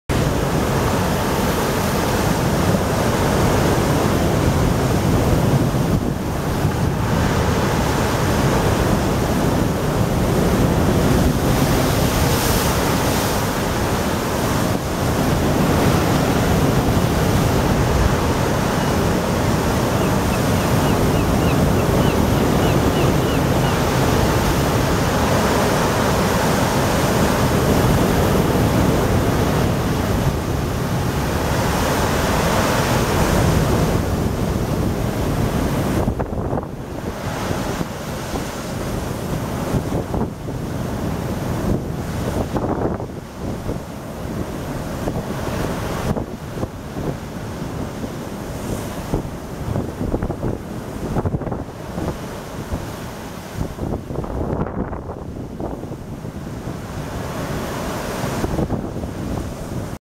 Blackpool. Salt splash. Hd.
Gale force winds and high seas batter the coastline of Blackpool.
Video taken from the safety of the south pier on 08/11/2013.